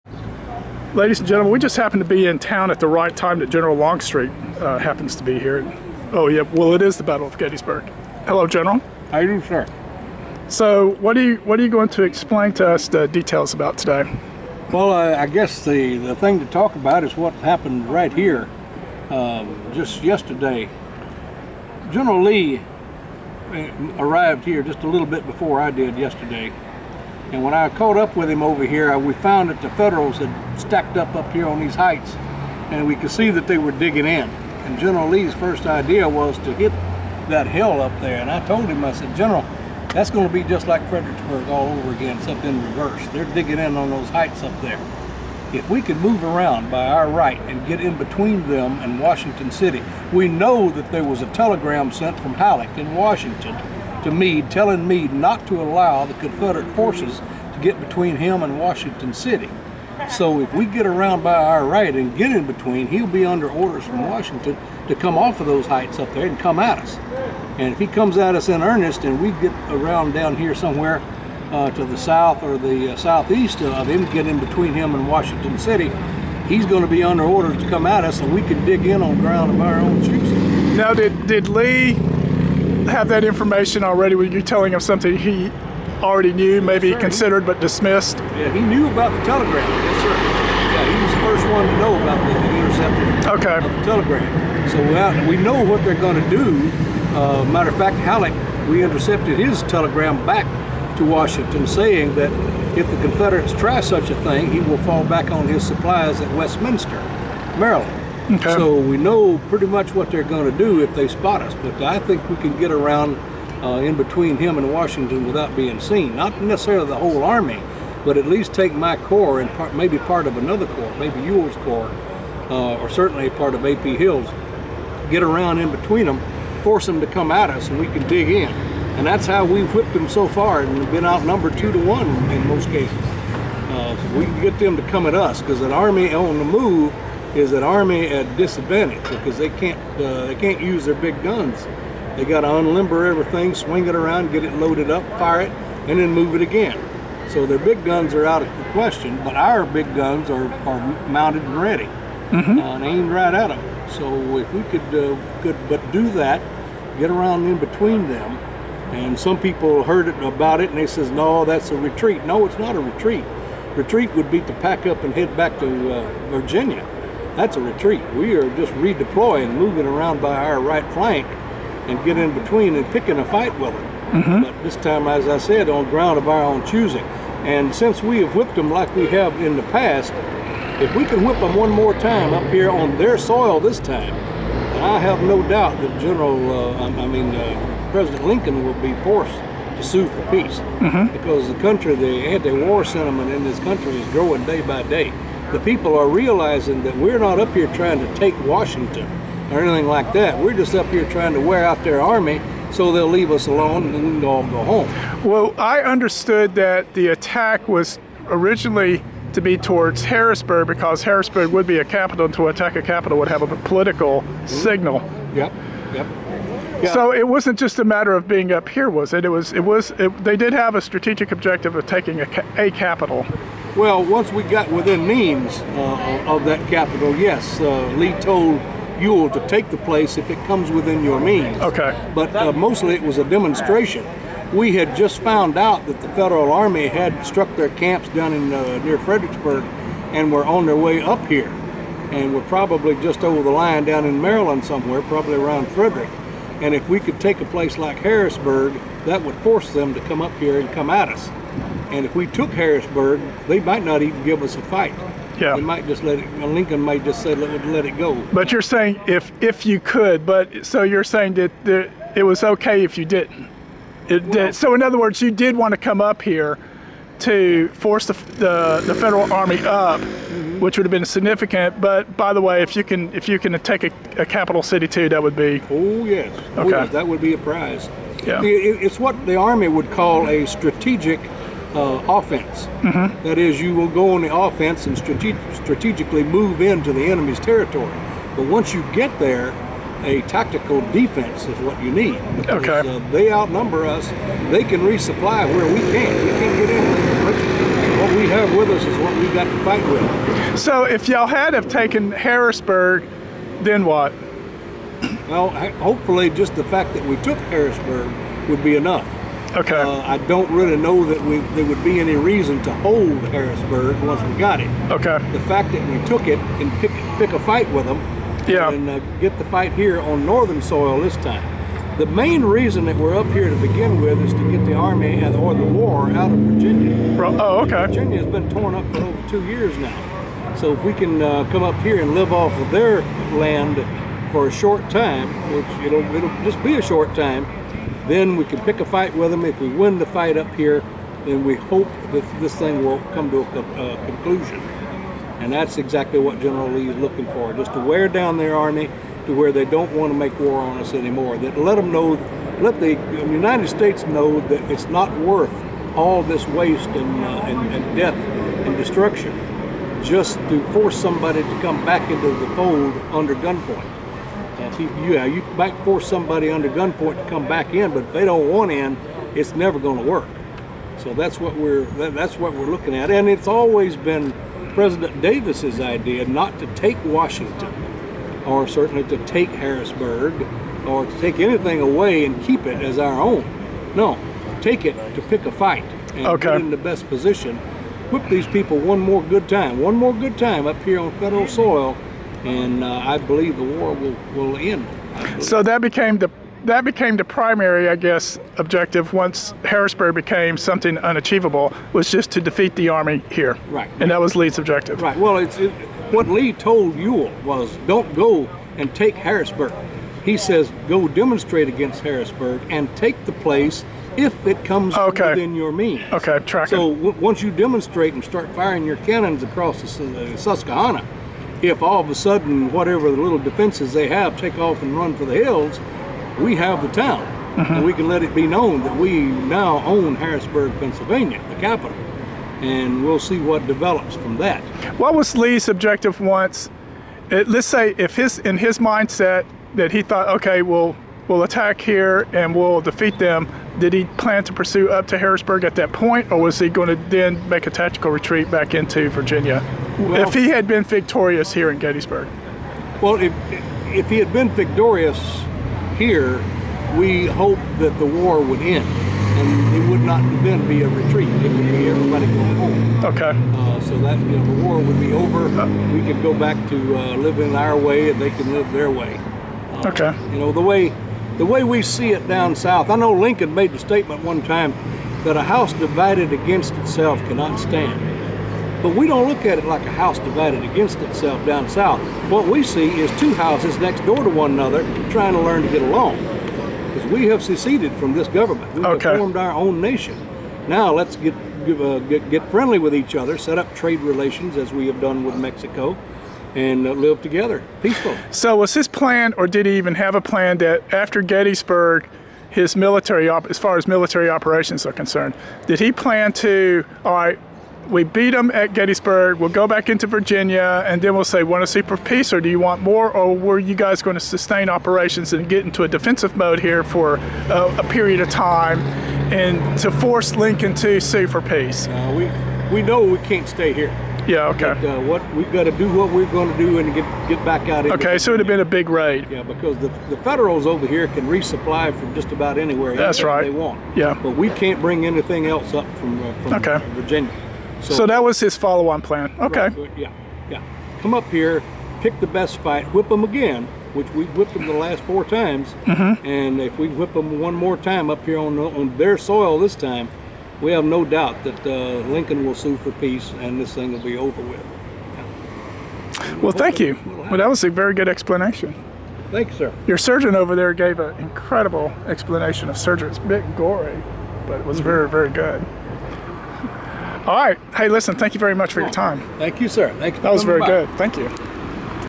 Gettysburg 2015: living historian: General James Longstreet
General Longstreet explains the battle of Gettysburg 2015